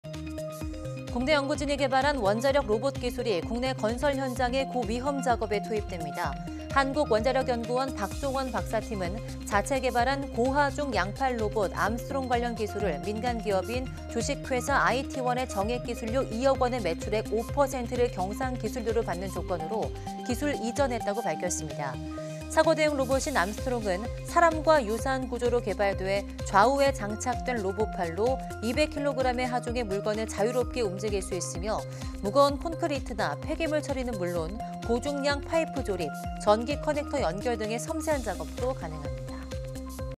원자력 로봇 기술, 고위험 건설 작업 대신한다 / YTN 사이언스
국내 연구진이 개발한 원자력 로봇 기술이 국내 건설 현장의 고위험 작업에 투입됩니다.

한국원자력연구원 박종원 박사팀은 자체 개발한 고하중 양팔 로봇 '암스트롱' 관련 기술을 민간기업인 (주)아이티원에 정액 기술료 2억 원에 매출액 5%를 경상 기술료로 받는 조건으로 기술 이전했다고 밝혔습니다.

사고 대응 로봇인 암스트롱은 사람과 유사한 구조로 개발돼 좌우에 장착된 로봇팔로 200kg 하중의 물건을 자유롭게 움직일 수 있으며, 무거운 콘크리트나 폐기물 처리는 물론 고중량 파이프 조립, 전기 커넥터 연결 등의 섬세한 작업도 가능합니다.